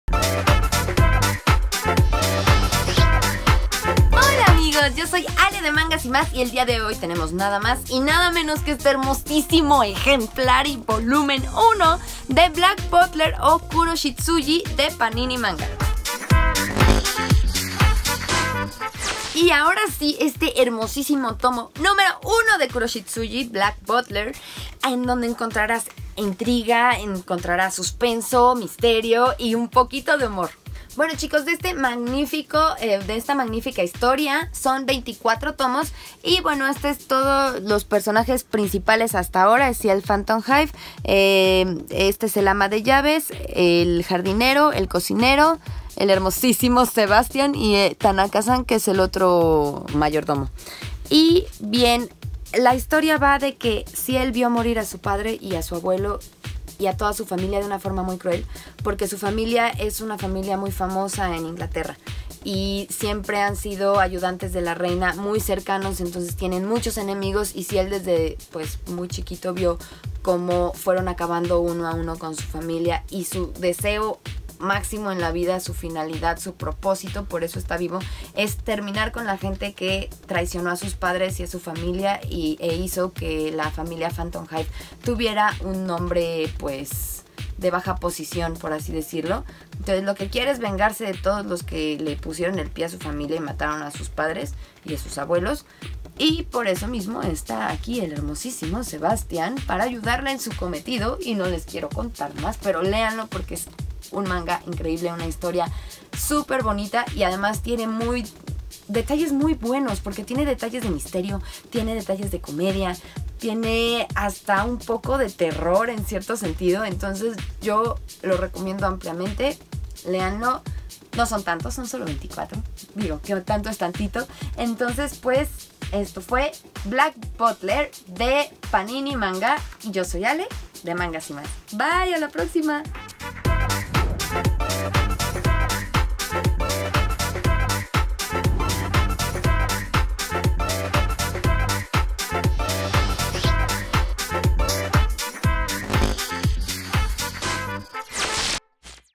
Manga y mas BLACK BUTLER #1 de Panini Manga ★ juegos juguetes y coleccionables ★
Manga y mas en este capitulo platica sobre el #1 de BLACK BUTLER
de Panini Manga Mexico